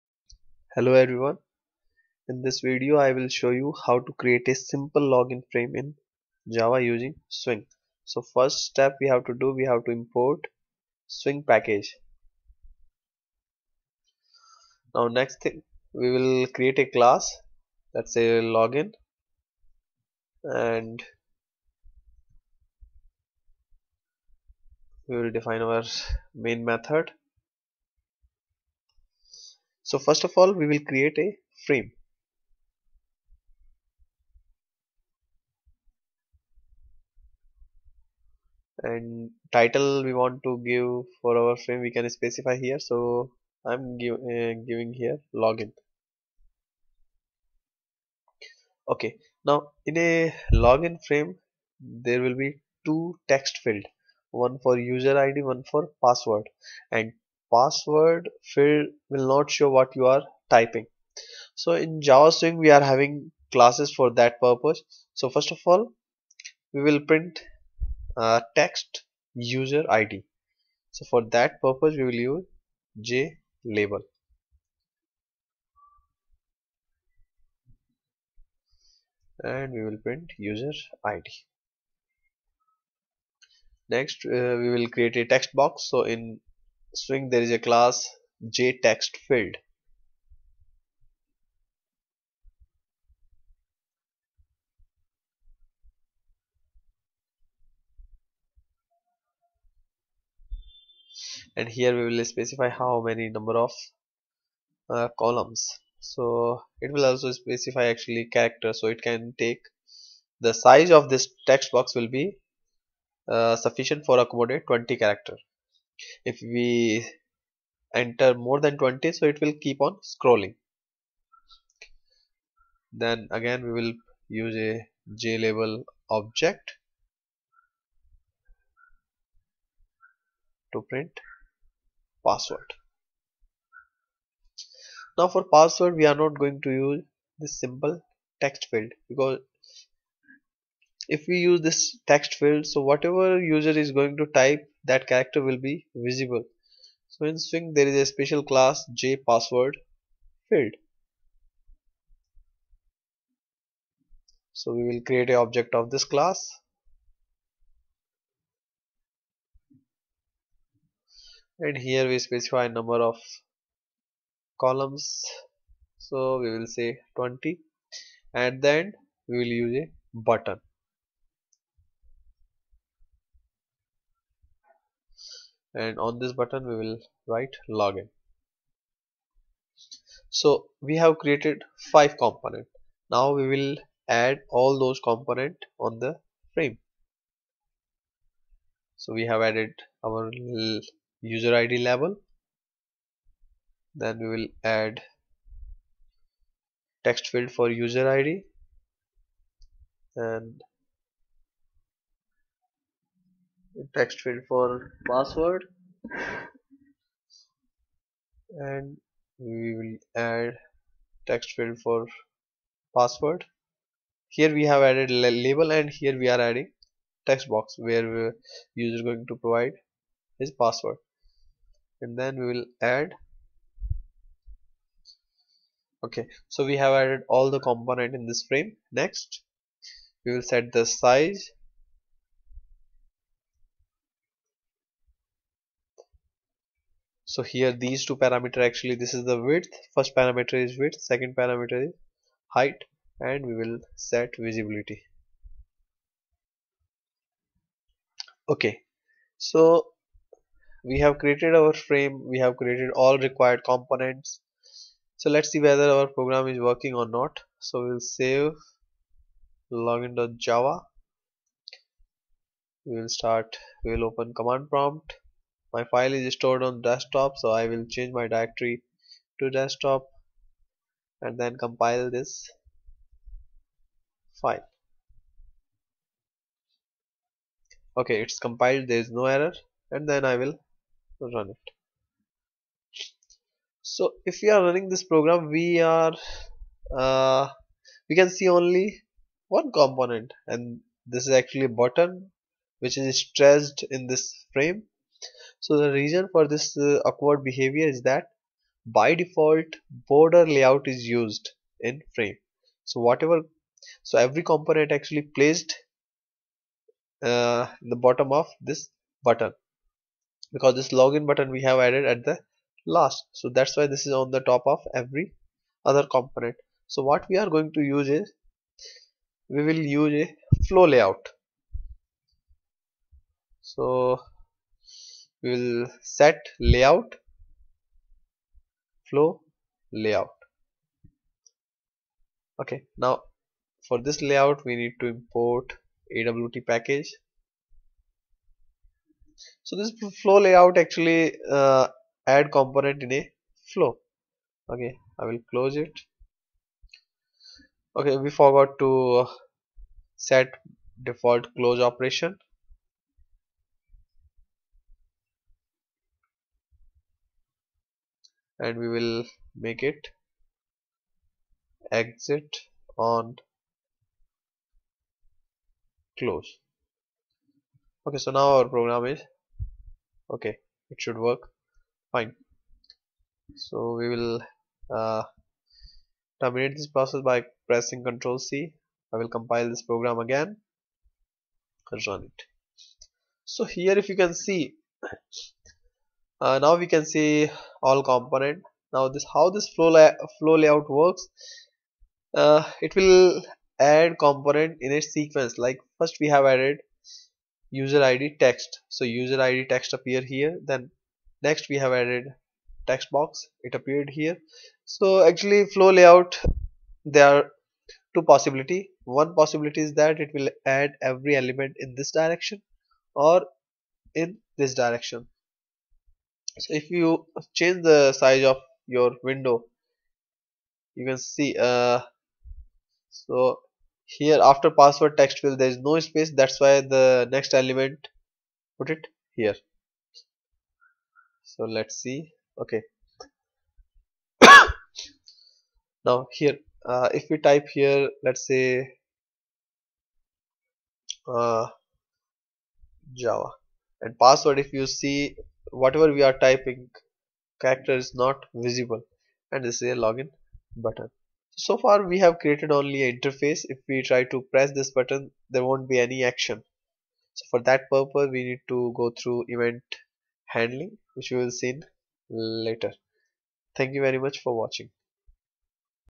How to Create a Simple Login Frame in Java Using Swing
Login frame is frequently developed exercise in java. In this video we will learn how we will develop a simple login frame with password field.